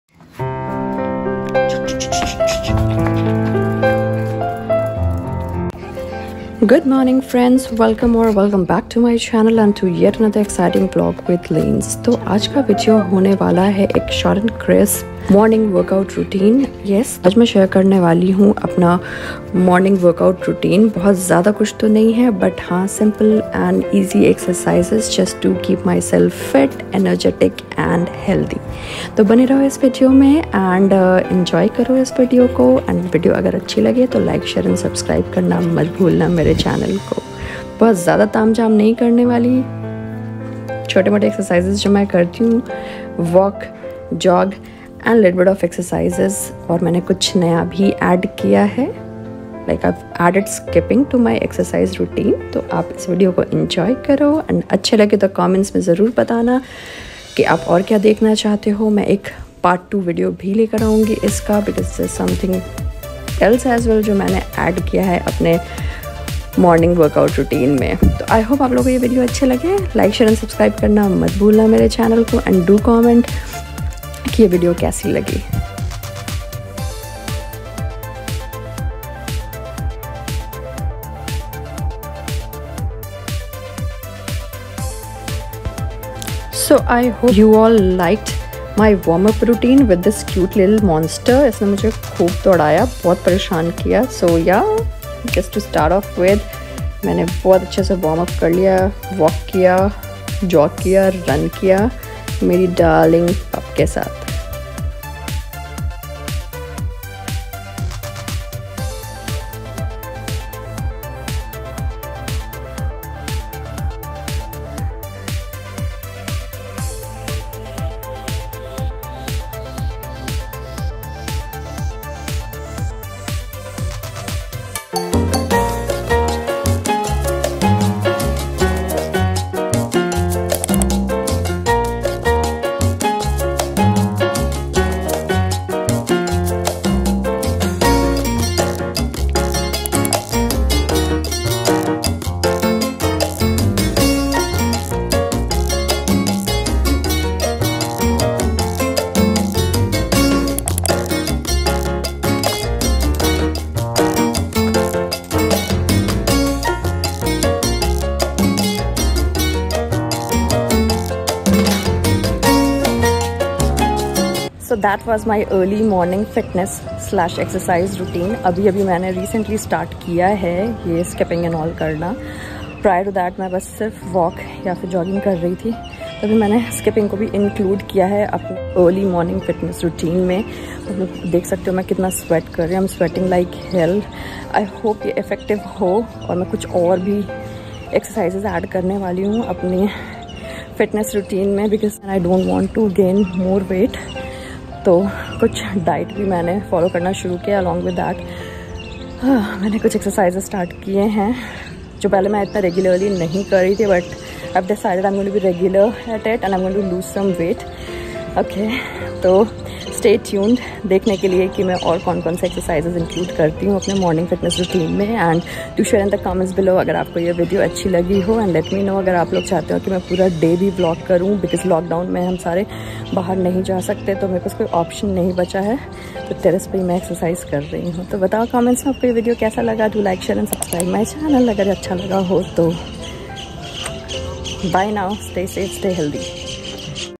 Morning Workout Routines At Home 2021 | Skipping & Stretching | Lockdown Edition
Hi All,
Sharing here my morning workout routine, a short & crisp video giving you all an idea the kind of workout & exercises I include in my day today life. I have been practicing yoga & meditation as well as a daily fitness routine of mine. Sharing here the links of some of the other vlogs featuring the same.